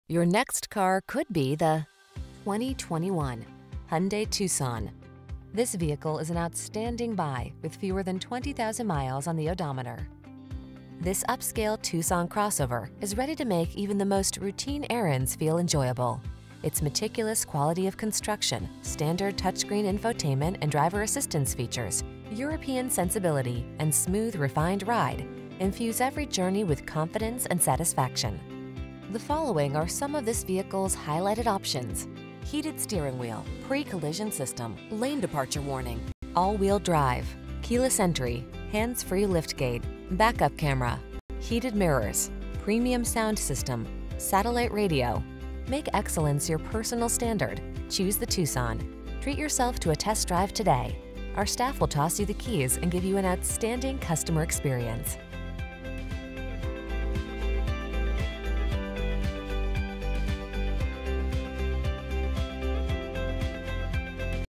2021 Hyundai Tucson Allentown, Coopersburg, Macungie, Easton, Nazereth 323687A
Magnetic Force Used 2021 Hyundai Tucson Limited available in 675 State Ave., Emmaus, Pennsylvania at Lehigh Valley Hyundai. Servicing the Allentown, Coopersburg, Macungie, Easton, Nazereth area.

Only 16754 Miles! Delivers 26 Highway MPG and 21 City MPG! This Hyundai Tucson boasts a Regular Unleaded I-4 2.4 L/144 engine powering this Automatic transmission. OPTION GROUP 01 MAGNETIC FORCE FIRST AID KIT.*This Hyundai Tucson Comes Equipped with These Options *CARGO COVER BLACK LEATHER SEAT TRIM ALL WEATHER FLOOR MATS Wheels: 18 x 7.0J Alloy Variable Intermittent Wipers w/Heated Wiper Park Valet Function Trunk/Hatch Auto-Latch Trip computer Transmission: 6-Speed Automatic w/SHIFTRONIC -inc: drive mode select Tires: 225/55R18.